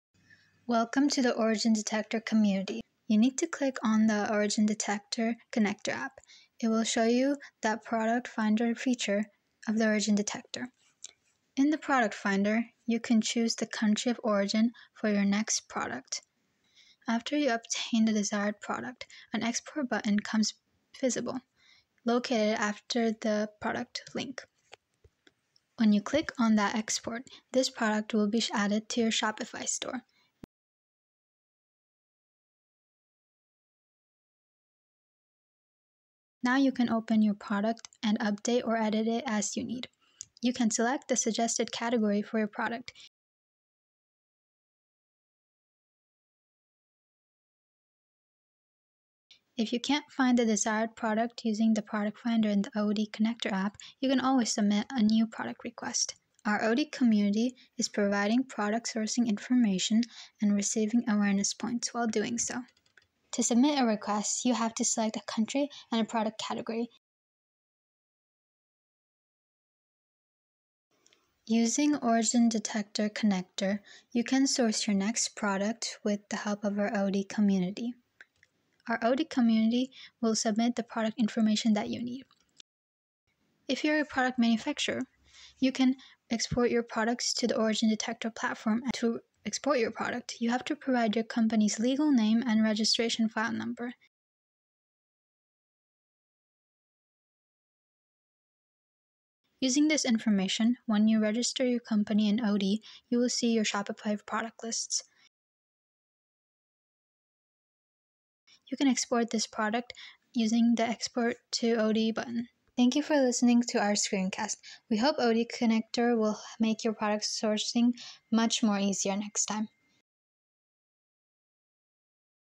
Use Origin Detector Connector app to source your next product in Shopify store
This screen cast shows how to import products in your Shopify store. It also explains how to add products in the Origin Detector platform as a manufacturer.